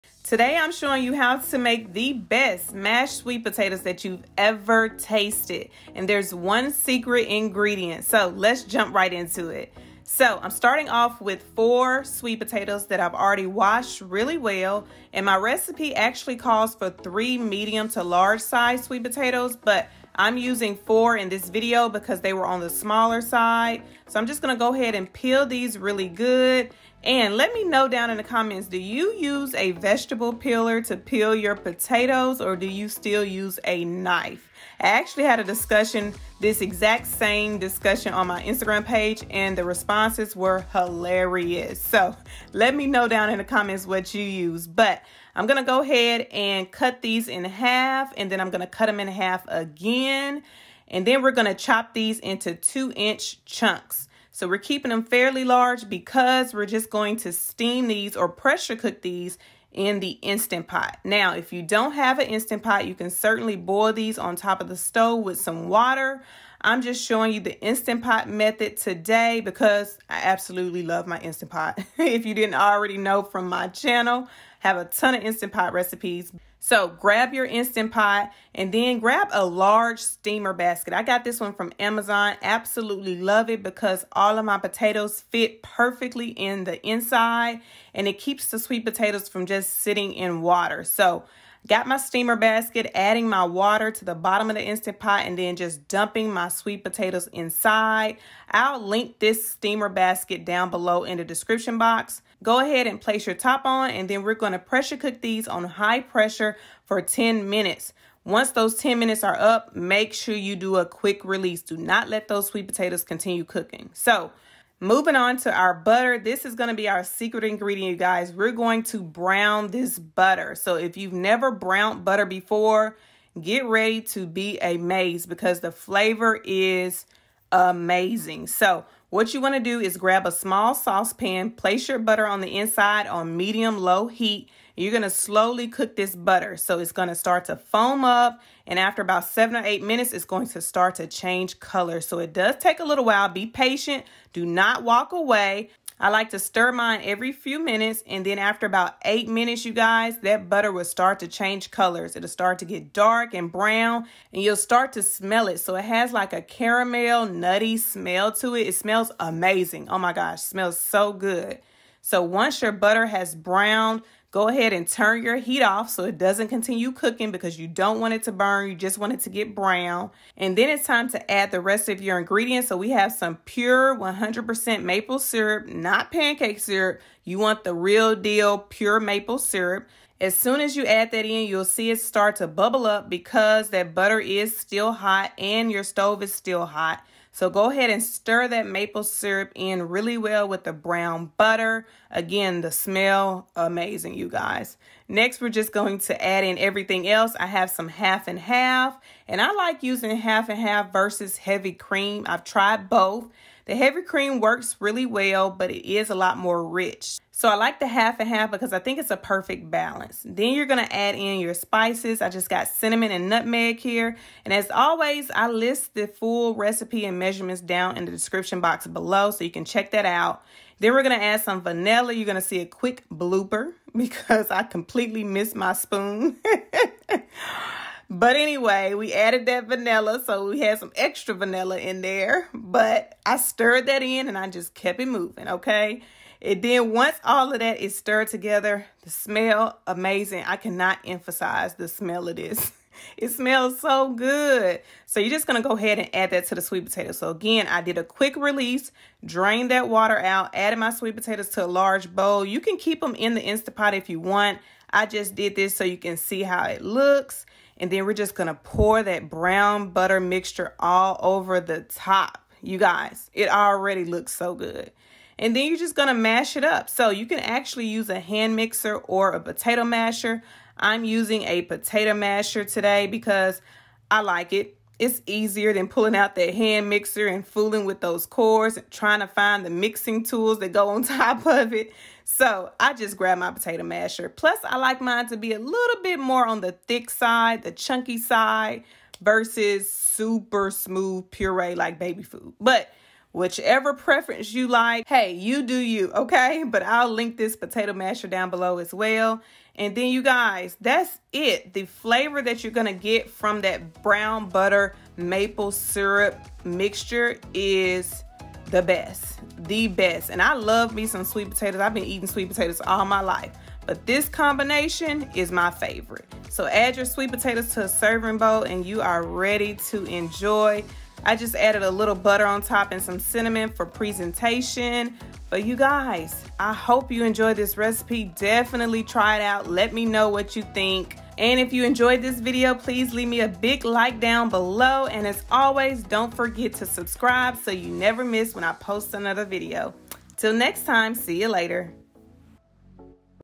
The Secret Ingredient for the BEST Sweet Potatoes | Instant Pot Mashed Sweet Potatoes!
These Instant Pot Brown Butter Mashed Sweet Potatoes are creamy, comforting and the perfect side dish to any meal. The combination of brown butter + pure maple syrup is the secret ingredient and it gives these sweet potatoes incredible flavor! You can make this easily in the Instant Pot or on the Stovetop. I’ve included both instructions in the recipe post below.

ITEMS USED IN THIS VIDEO:
Potato Masher
Steamer Basket
Instant Pot